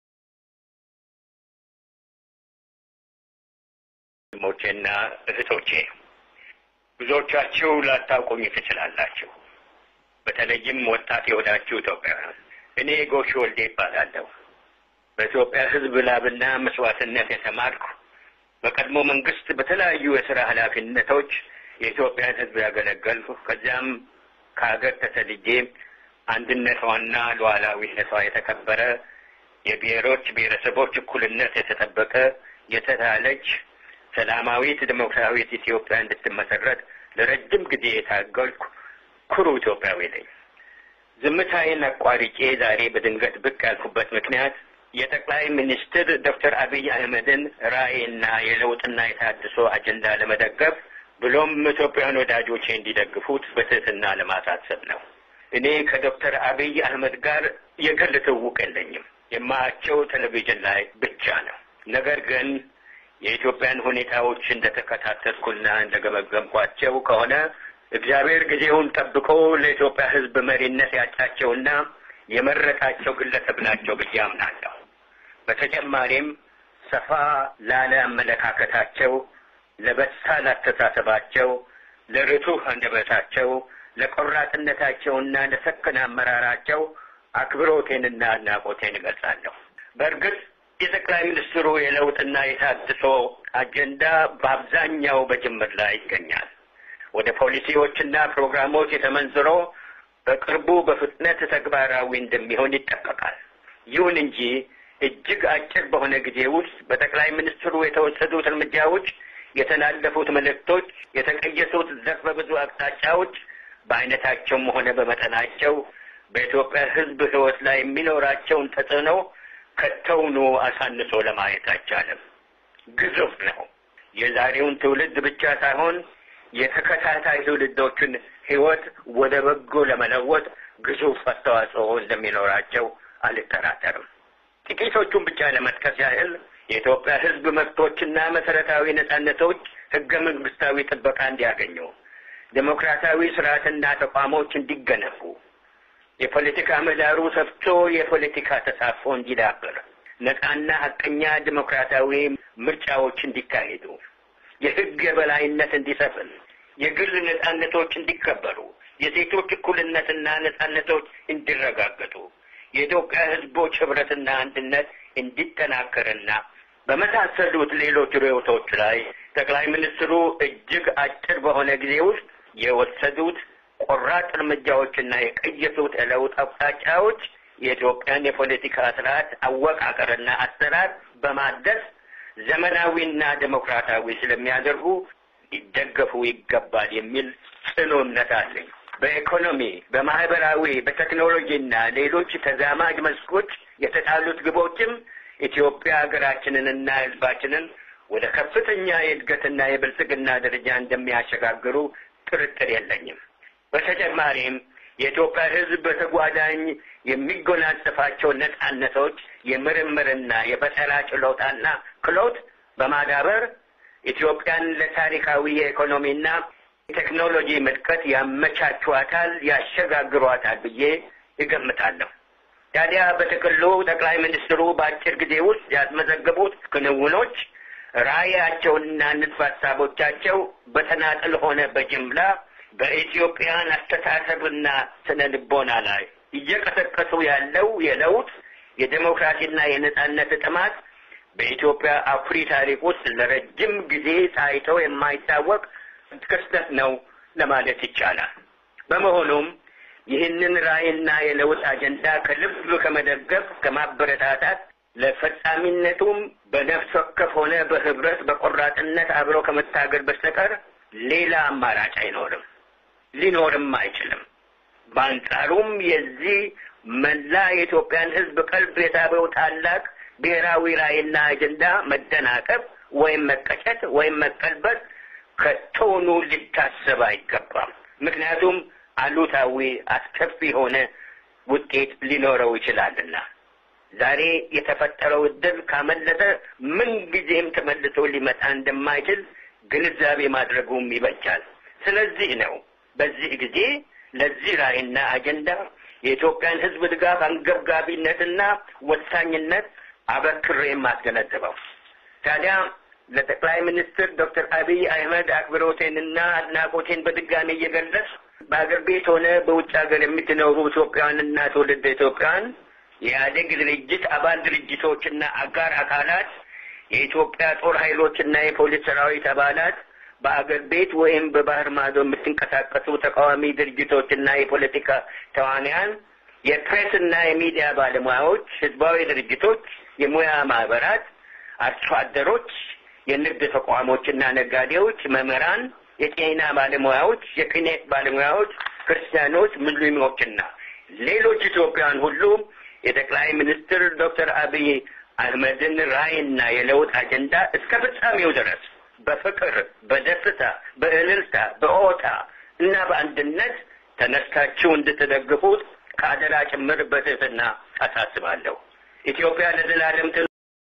የካረኔል ጉሻ ወልዴ መመልከት
የካረኔል ጉሻ ወልዴ መልካት ለጠቅላዬ አሰተላለፎ ከአሜሪካ በድምፅ ለመላው ኢትዬጲያውያን ጨምሮ